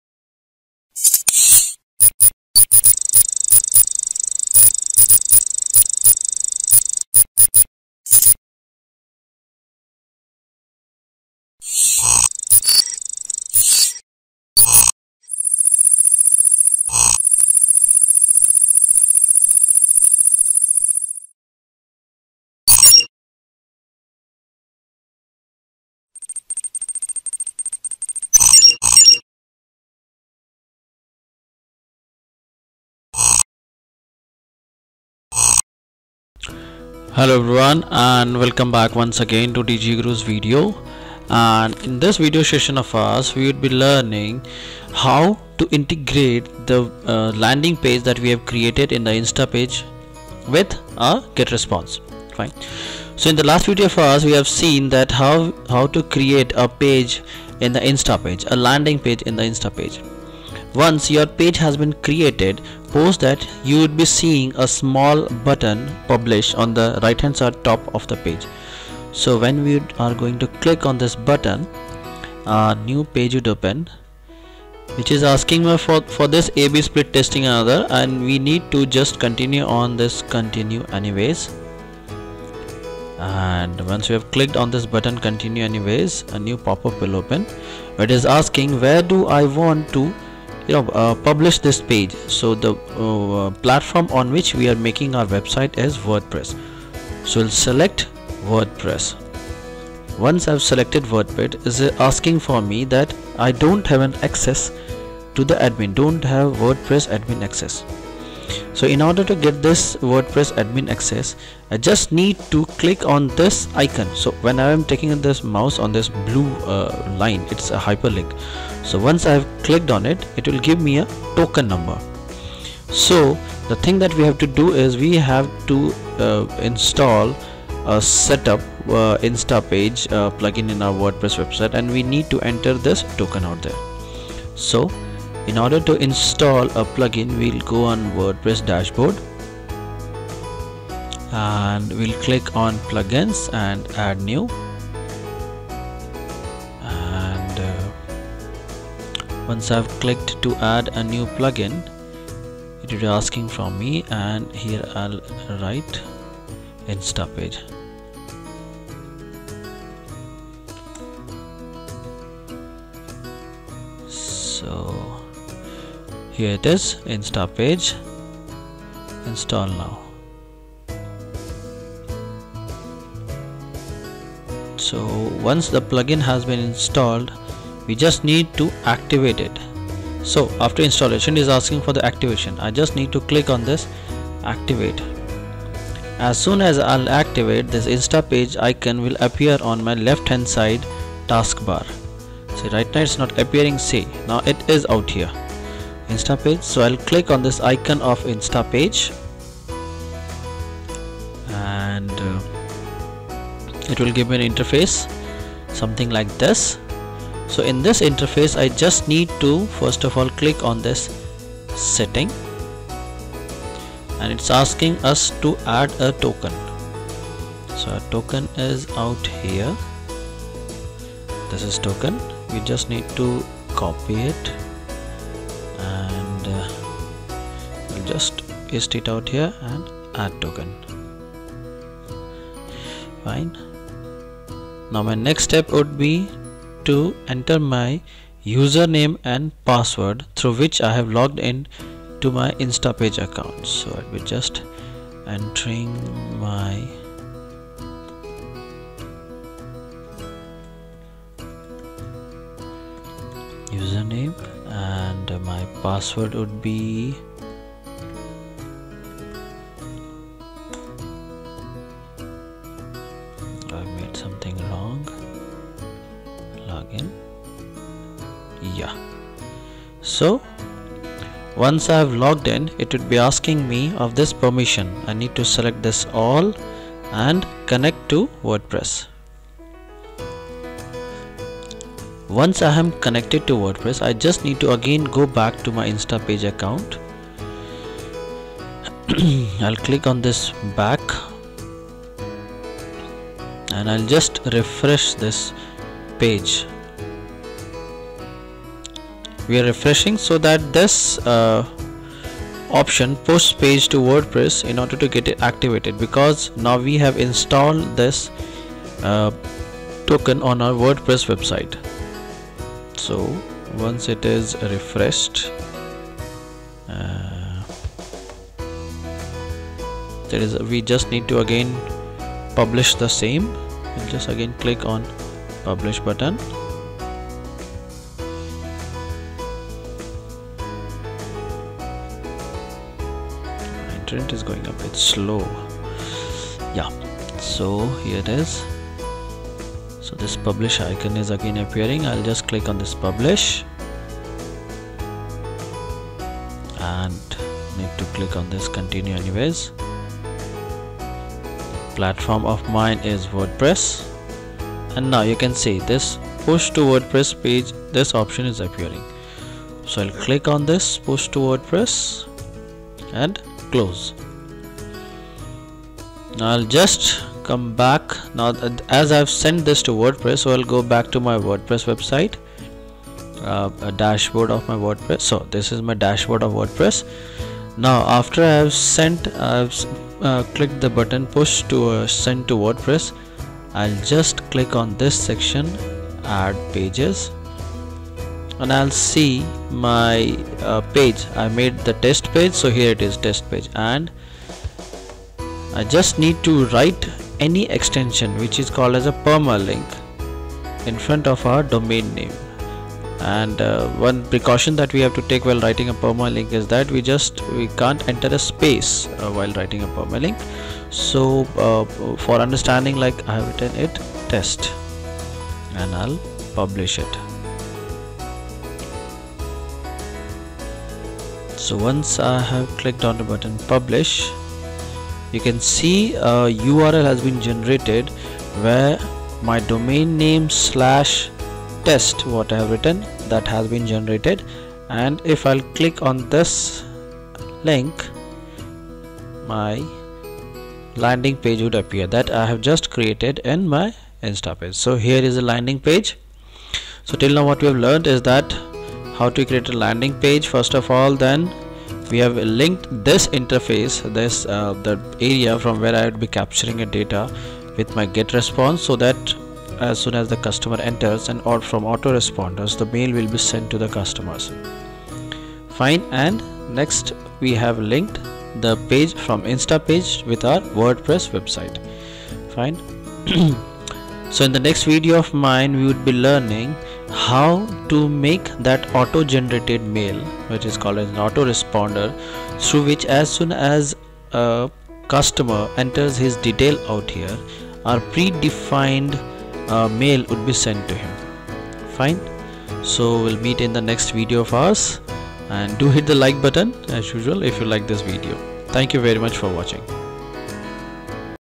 How to integrate Instapage lead generation page with Wordpress 2017
In this video we will learn how t integrate the form field in instapage with getresponse so that as soon as customer will enter the data in the instapage a mail will be sent from Getresponse